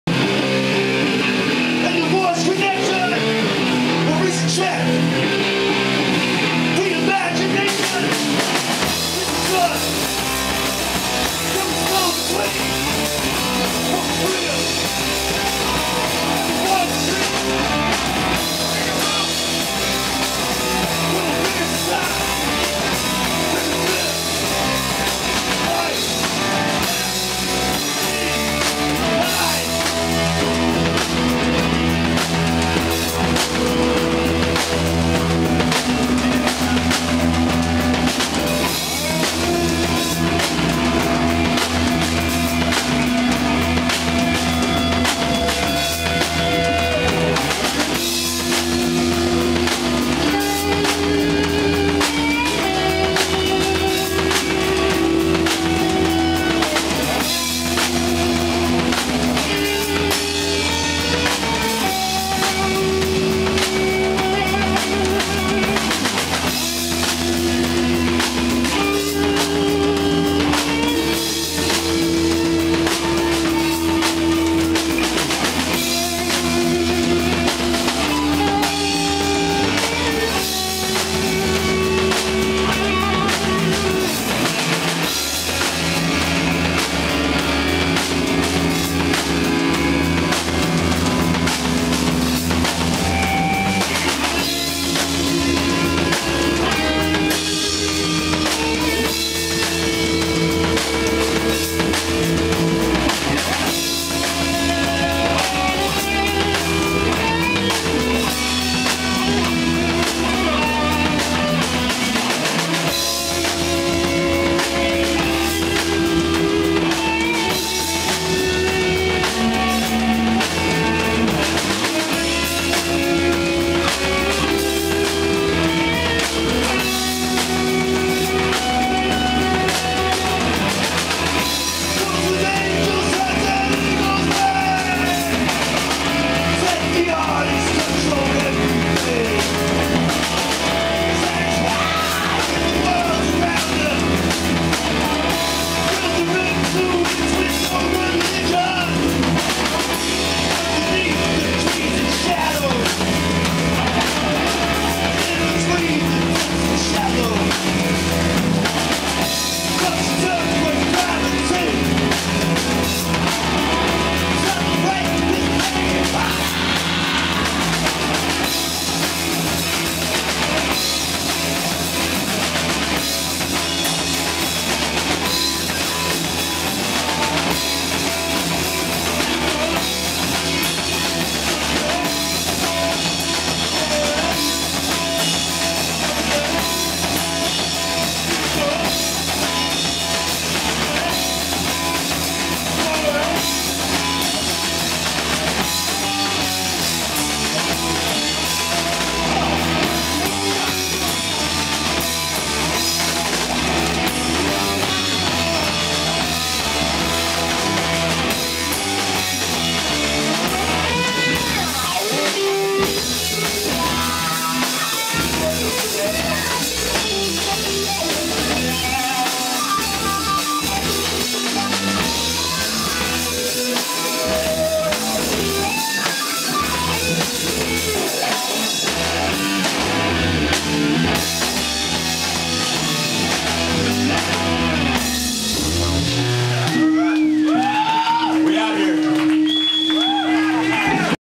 War Child at Thee Parkside
War Child's first show at Thee Parkside. A Benefit show for Flowershop DIY Skatepark.  August 11th, 2011.